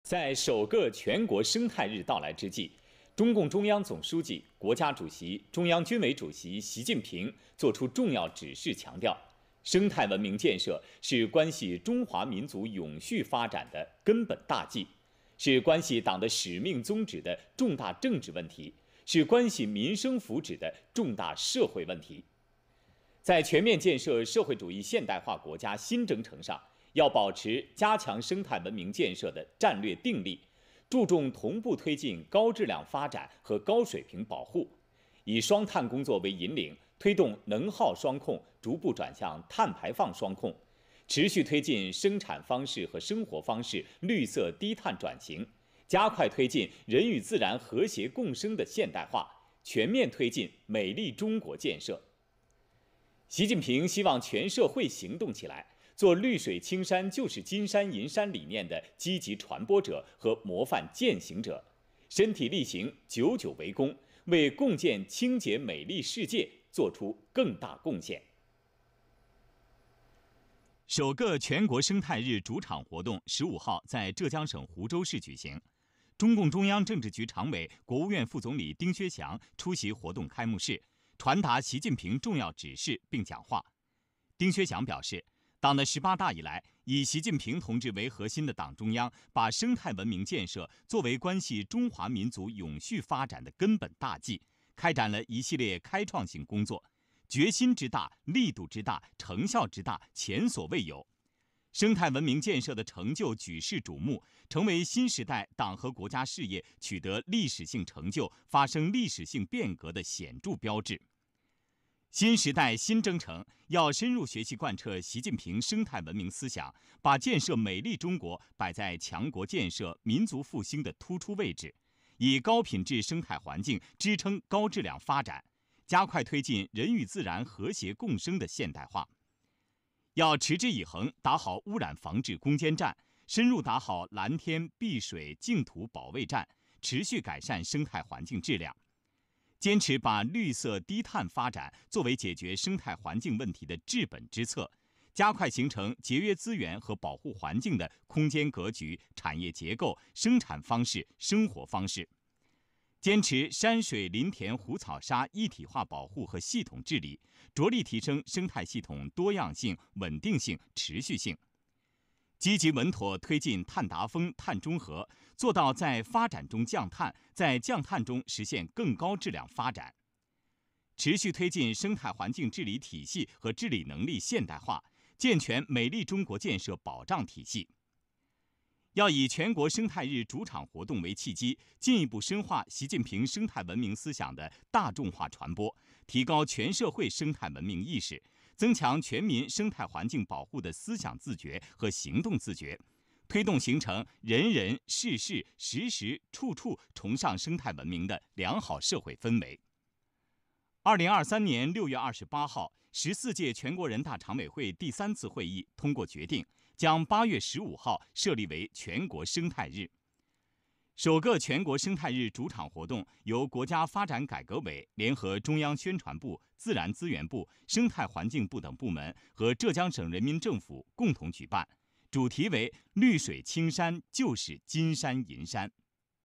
习近平在首个全国生态日之际作出重要指示强调 全社会行动起来做绿水青山就是金山银山理念的积极传播者和模范践行者
在首个全国生态日到来之际，中共中央总书记、国家主席、中央军委主席习近平作出重要指示强调，生态文明建设是关系中华民族永续发展的根本大计，是关系党的使命宗旨的重大政治问题，是关系民生福祉的重大社会问题。在全面建设社会主义现代化国家新征程上，要保持加强生态文明建设的战略定力，注重同步推进高质量发展和高水平保护，以“双碳”工作为引领，推动能耗双控逐步转向碳排放双控，持续推进生产方式和生活方式绿色低碳转型，加快推进人与自然和谐共生的现代化，全面推进美丽中国建设。
习近平希望全社会行动起来，做绿水青山就是金山银山理念的积极传播者和模范践行者，身体力行、久久为功，为共建清洁美丽世界作出更大贡献。